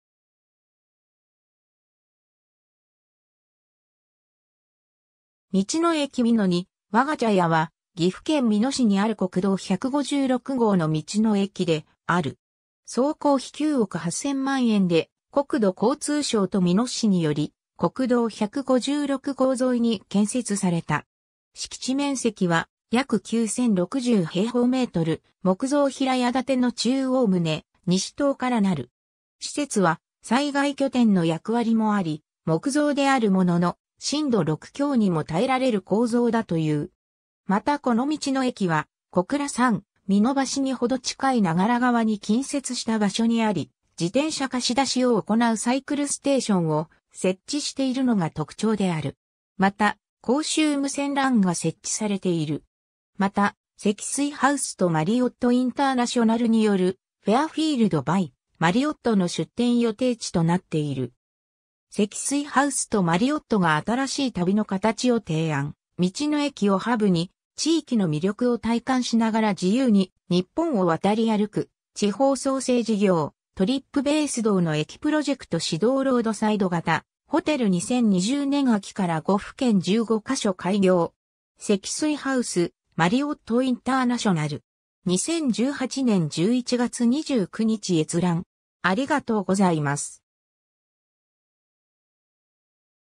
道の駅美濃にわか茶屋
道の駅美濃にわか茶屋（みちのえき みのにわかちゃや）は、岐阜県美濃市にある国道156号の道の駅である。
 総工費9億8,000万円で国土交通省と美濃市により国道156号沿いに建設された。
敷地面積は約9,060 m2、木造平屋建の中央棟、西棟からなる。
施設は災害拠点の役割もあり、木造（長良杉を使用）であるものの、震度6強にも耐えられる構造だという。
またこの道の駅は小倉山、美濃橋に程近い長良川に近接した場所にあり、自転車貸し出しを行うサイクルステーションを設置しているのが特徴である。
また公衆無線LAN（FREESPOT）が設置されている。
 また、積水ハウスとマリオット・インターナショナルによる「フェアフィールド・バイ・マリオット」の出店予定地となっている。
 ^ “積水ハウスとマリオットが新しい旅のカタチを提案　「道の駅」をハブに、地域の魅力を体感しながら自由にニッポンを渡り歩く　地方創生事業「Trip Base 道の駅プロジェクト」始動　ロードサイド型ホテル 2020年秋から5府県15カ所開業 (PDF)”. 積水ハウス、マリオット・インターナショナル (2018年11月28日). 2018年11月29日閲覧。